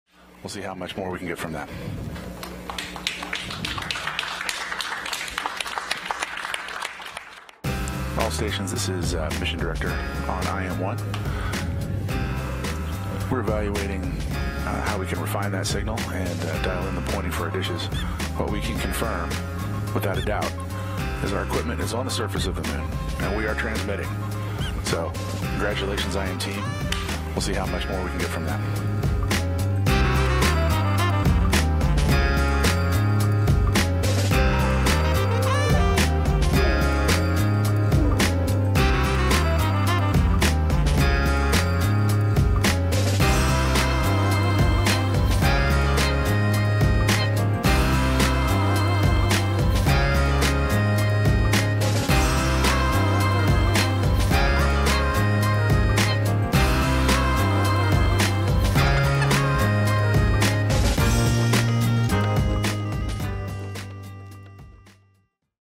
Una nave comercial con bandera de EE. UU. logra una hazaña y aluniza | El Espectador
La nave Odysseus (u Odiseo, en español) ha logrado alunizar con éxito en el polo sur del satélite. De la mano de una empresa privada, llamada Intuitive Machines, Estados Unidos vuelve a poner un artefacto con su bandera, después de medio siglo desde la última misión Apolo.